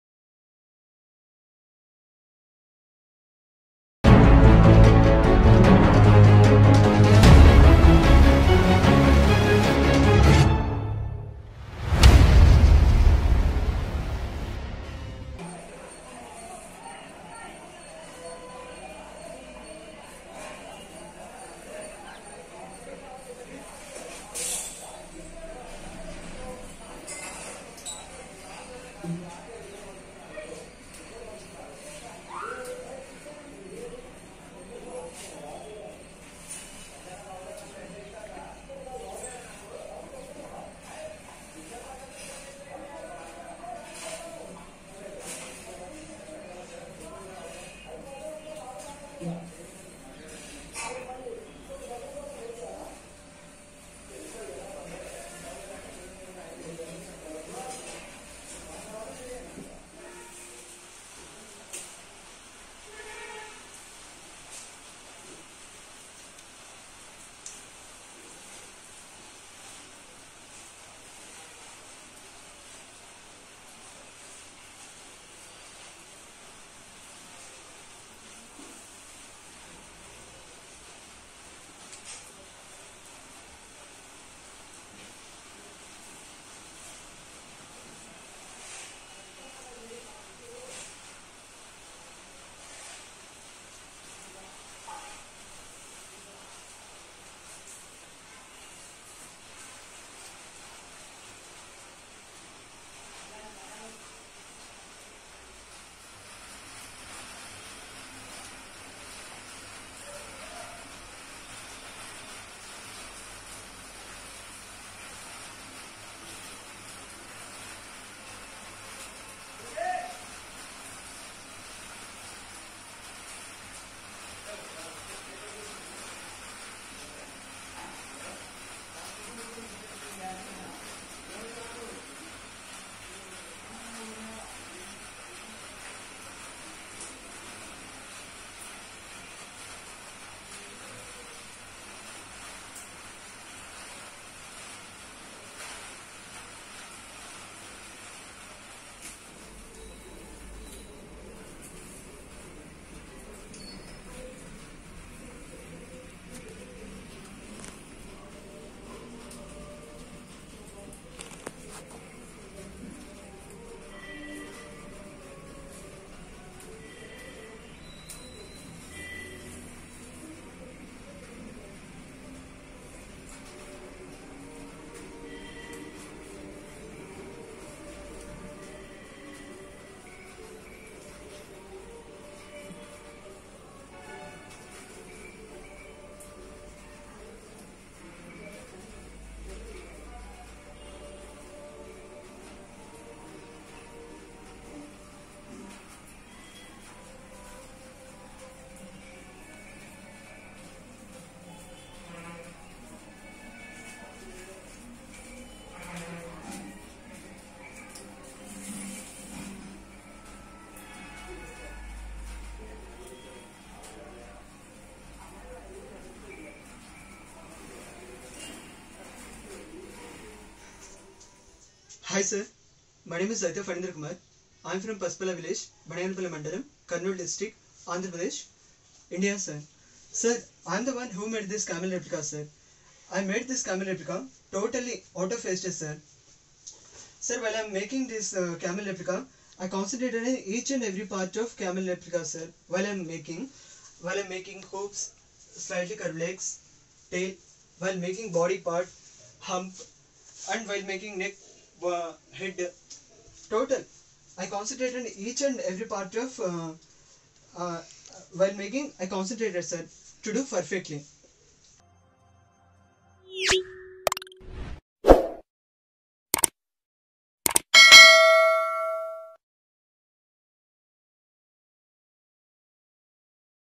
Largest replica of camel using empty cigarette packets
The record for making the largest replica of the camel using empty cigarette packets was set by Itha Fanindra Kumar (born on June 12, 1996) of Kurnool, Andhra Pradesh. He made the replica measuring 7 feet and 1 inch in height, 4 feet and 5 inches in width using 1,287 empty cigarette packets and waste materials, as confirmed on November 1, 2021.